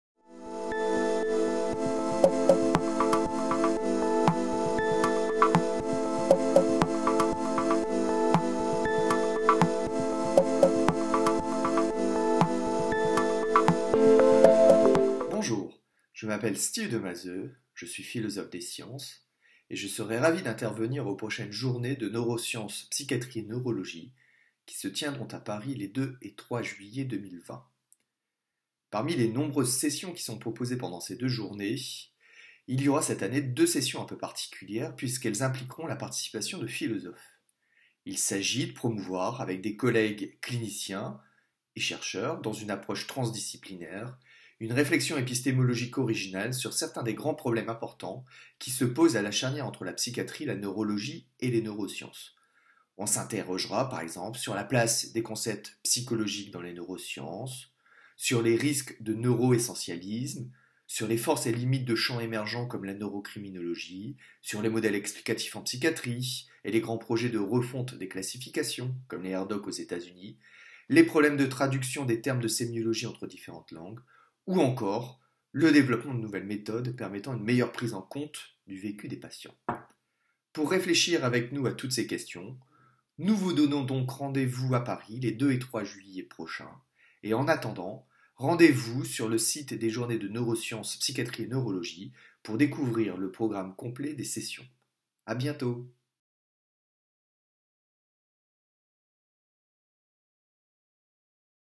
Les mots de la psychiatrie, quelles confusions ? Perspectives épistémologiques
Par Demazeux Steeves, MCF philosophie, Université Bordeaux-Montaigne (Laboratoire SPH EA 4574) : « Le projet RDoC et son grand chambardement de la sémiologie psychiatrique ».
 « La séparation disciplinaire entre psychiatrie et neurologie est un fait majeur du XXe siècle. Elle se justifie sans doute sur les plans clinique et thérapeutique, mais elle reste fragile sur le plan épistémologique, et d'autant plus fragile que les progrès des neurosciences démontrent le caractère inutile et caduque du dualisme métaphysique entre corps et esprit. Les journées Neurosciences, psychiatrie et neurologie ont pour but, en rassemblant des chercheurs, des cliniciens et des étudiants, de briser les cloisons disciplinaires et de tisser des liens nouveaux qui s'appuient sur les recherches scientifiques les plus foisonnantes en neurosciences. »
Projet Philo-CINAPs (Philosophy of Clinics In Neuroscience And Psychiatry), Programme Emergence IdEx, Université Bordeaux-Montaigne et Université de Bordeaux.

Les mots de la psychiatrie, quelles confusions ?
Perspectives épistémologiques
Président : S. Demazeux, Bordeaux
Le projet RDoC et son grand chambardement de la sémiologie psychiatrique
S. Demazeux, Bordeaux
Les ‘patients reported outcomes’ : donner la parole aux personnes concernées
L. Lecardeur, Nice
Impact de la littérature scientifique anglophone et de sa traduction
sur l’évolution du langage psychiatrique français
M-A Crocq, Rouffach